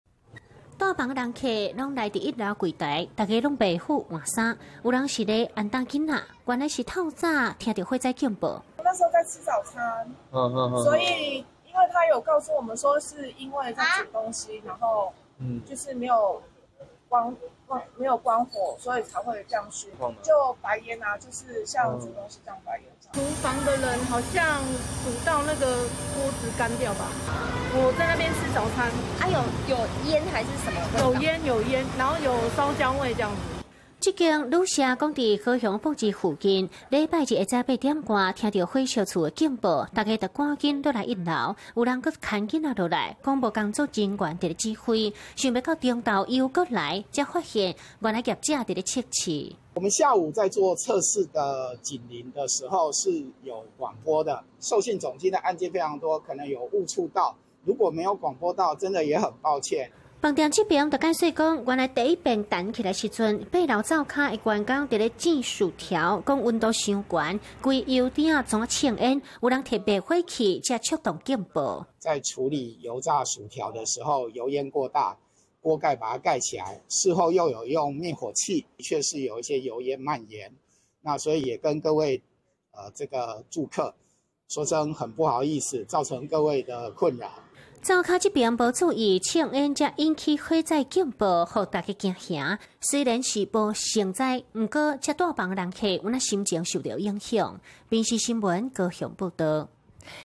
廚房準備早餐煙大警報響 旅館房客嚇壞驚慌逃生－民視台語新聞
【民視即時新聞】高雄鹽埕一間旅店，忽然火災警報大響，讓一大堆旅客抓著小孩往一樓衝，後來才發現，原來是廚房烹飪早餐，煙太大，誤觸警鈴，虛驚一場，沒想到，大家回房間後，又響第二次，才發現是旅店人員在測試，讓民眾又氣又無奈。